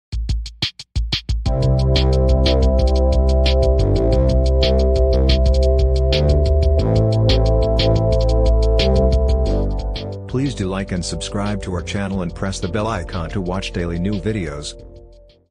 Dogs Love Towards Humans 😍
Humans and animals are best friends. In this video, we are going to share clips of dogs towards humans. Watch this amazing video, and you will find it hard to believe that these dogs could feel any more love or devotion to their human companions.